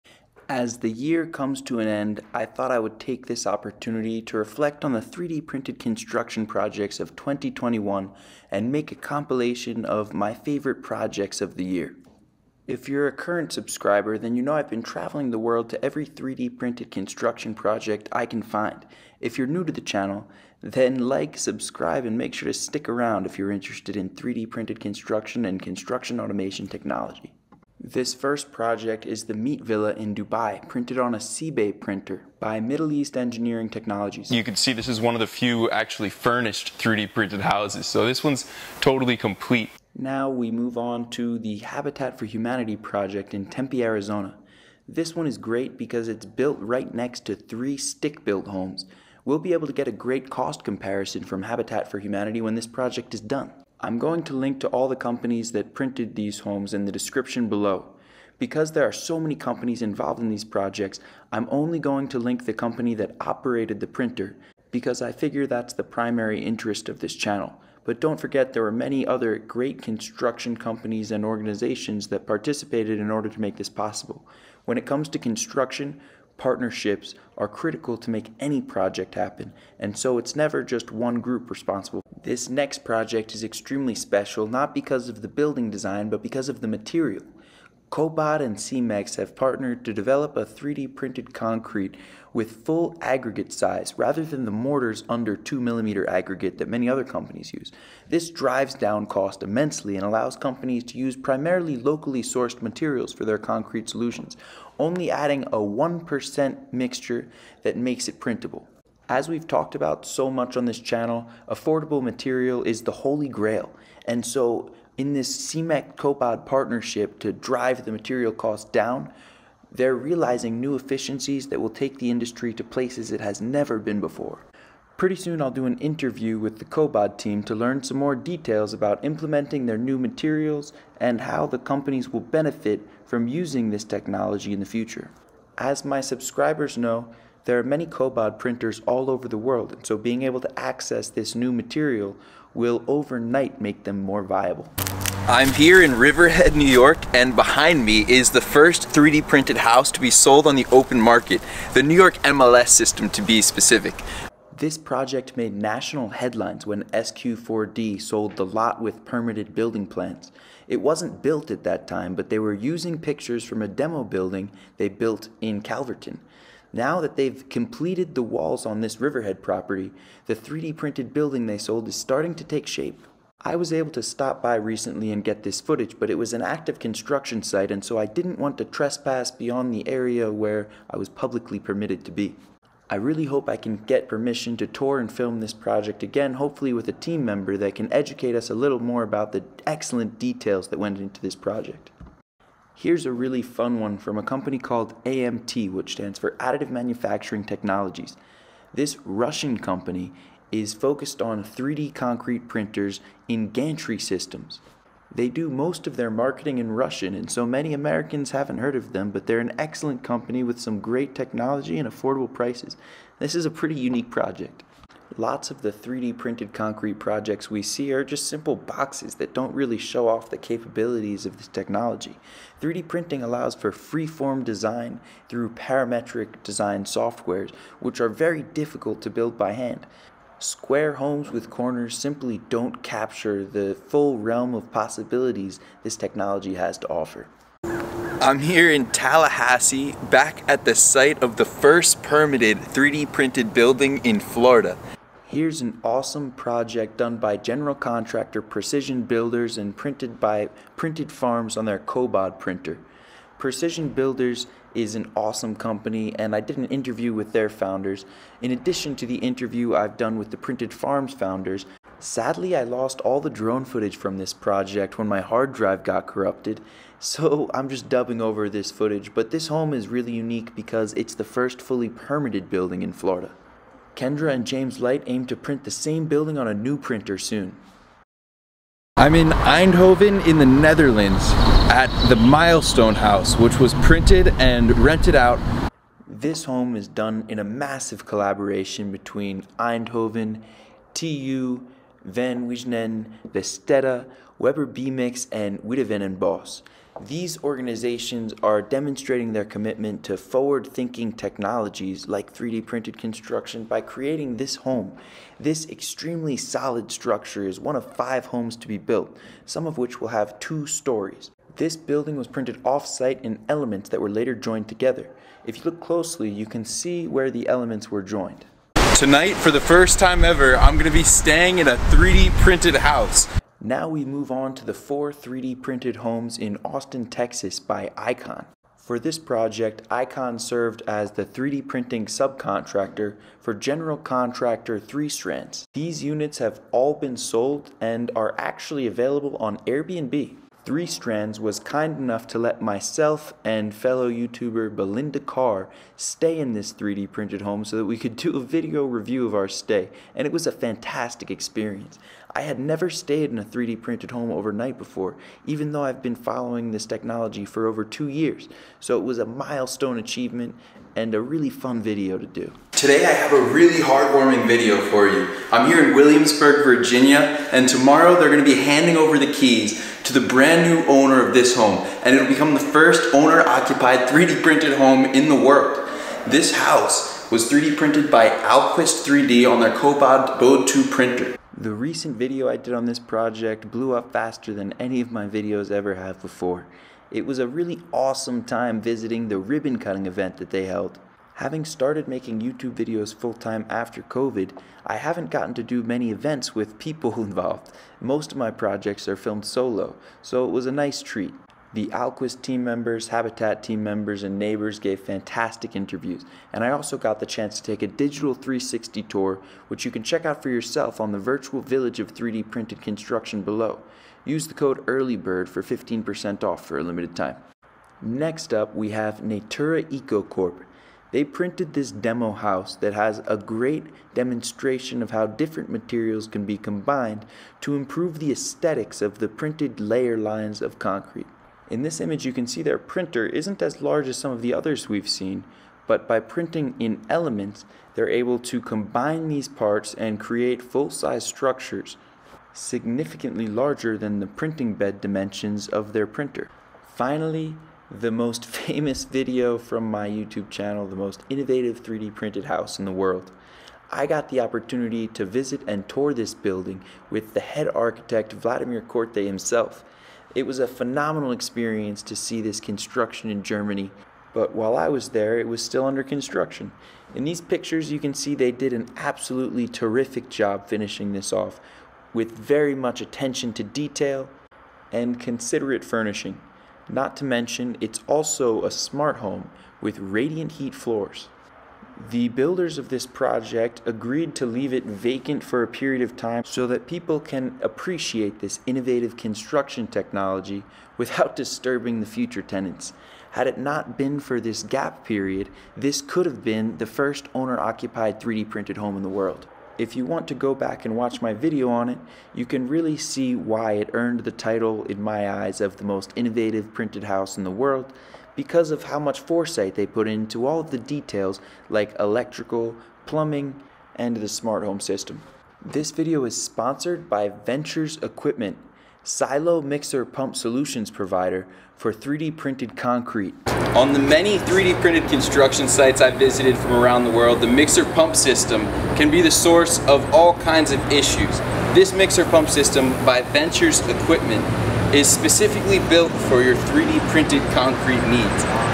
Top 10 3D Printed Houses of 2021
I went to 3D 3d printed construction projects in 8 countries including USA, Canada, Germany, France, Swizerland, Denmark, Belgium, and The Netherlands. I also visited 3D printed construction projects in 9 states, New York, New Jersey, Massachusetts, Virginia, Carolina, Florida, Texas, Arizona, and California (nearly one in Lousisiana too but it never happened).

Here are the top 10 houses 3D printed in 2021 in no particular order, some of which I have visited in person others I have not. All of these projects are specifically printed in 2021, to see projects from last year check out my video on the top 3D printed houses in 2020. I hope to continue this video at the end of every year! As more houses are printed I will have to become more exclusionary but for this year the conditions to enter were that the house has to have a roof, windows, electrical and either be a permitted structure or look convincingly complete. There were many other great projects this year that didn't quite fit that description.

Sign up for the "How to 3D Print a House" course available now at the beta pricing!

TOP 10 3D PRINTED HOUSES LINKS